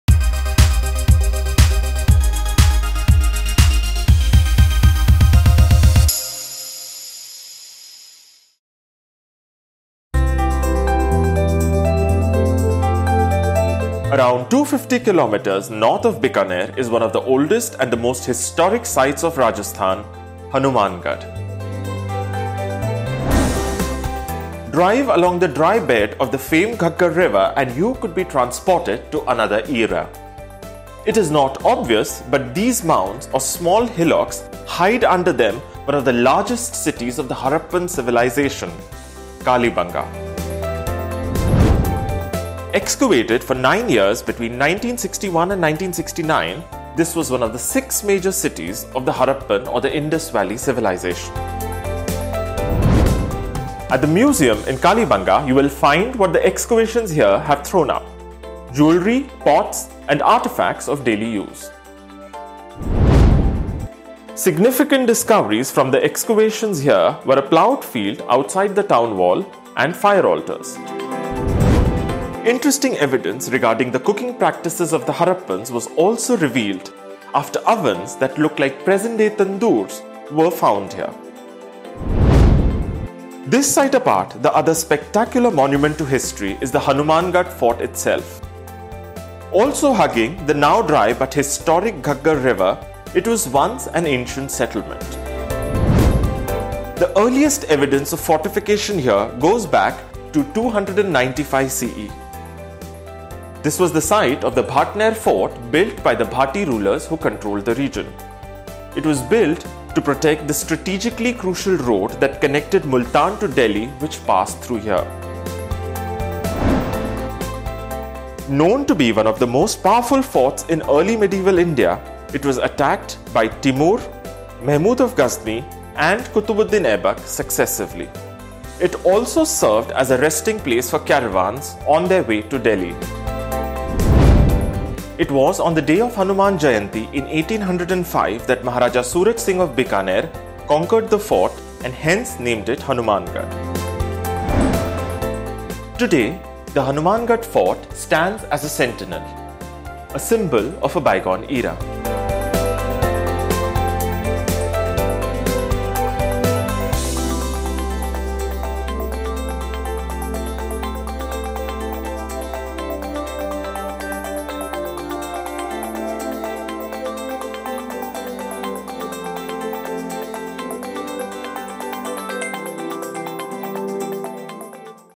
Hanumangarh: The Ancient Heart
It is one of the oldest and most historic cities of Rajasthan, but under the mounds and hillocks of Hanumangarh lie the remnants of an even older civilization, that of the lost city of the Harappans, Kalibangan. LHI digs deeper into the story behind this fascinating find.